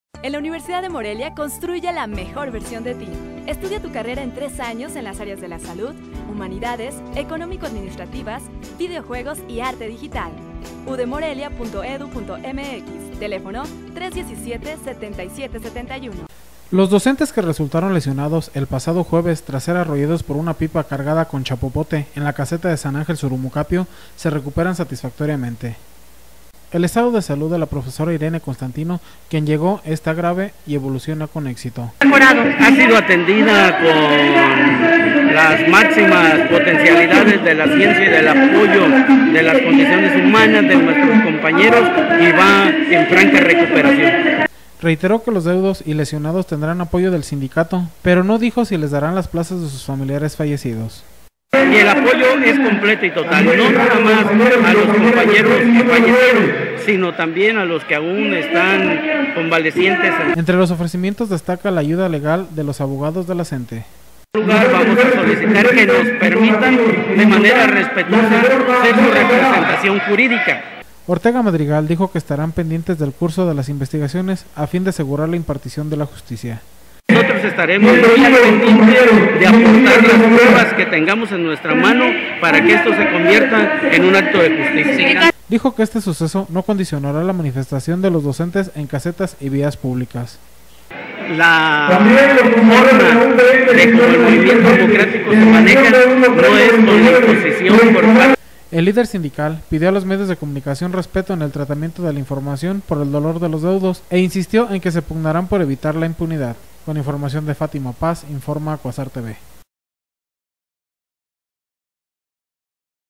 Se recuperan con éxito lesionados por el pipazo
Los docentes que resultaron lesionados el pasado jueves tras ser arrollados por una pipa cargada con chapopote en la caseta de San Ángel Zurumucapio, se recuperan satisfactoriamente...